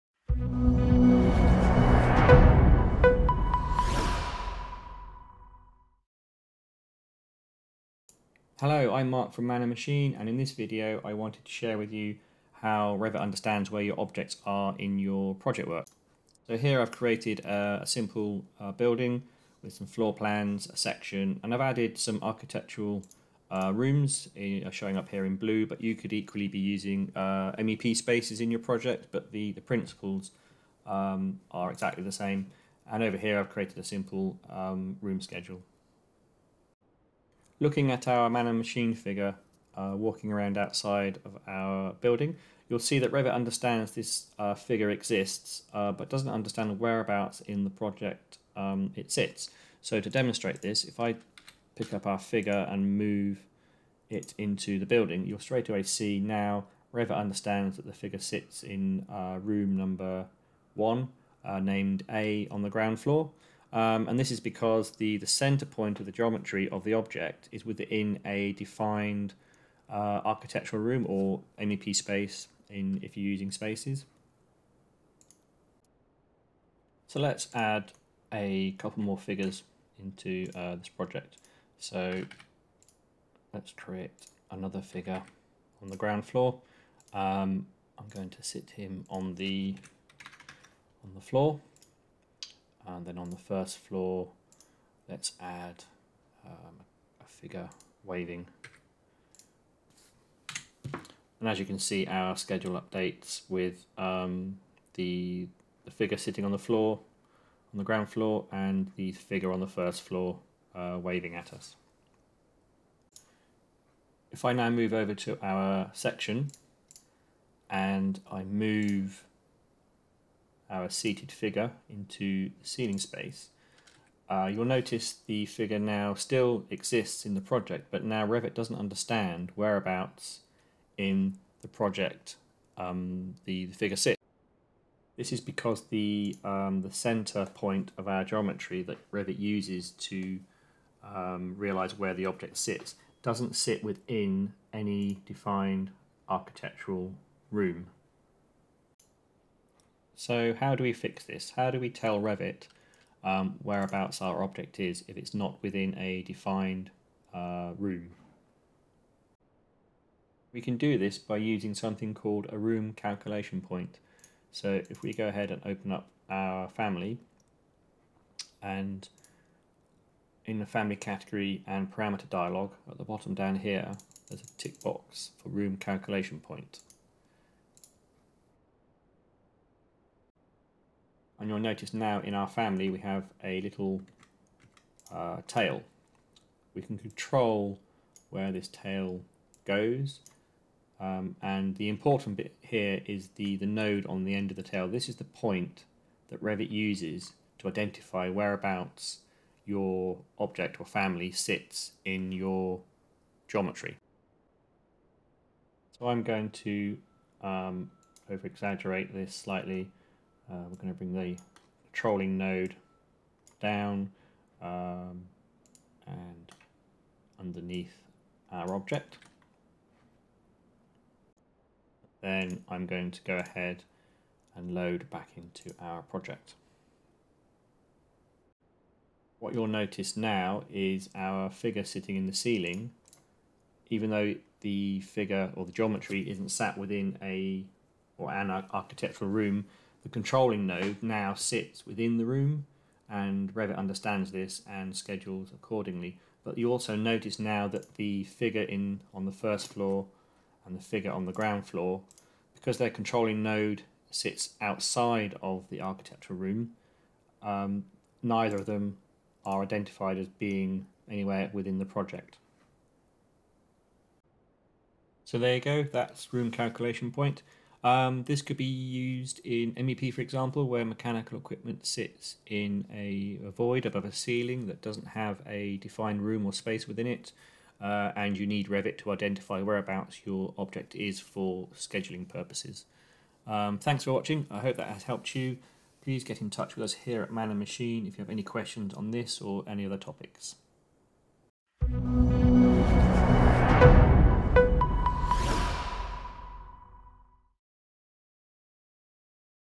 Autodesk Revit Room Calculation Point
How does Room Calculation Point work? This short video explains what it is and how it can help identify where your Revit families are in your project work.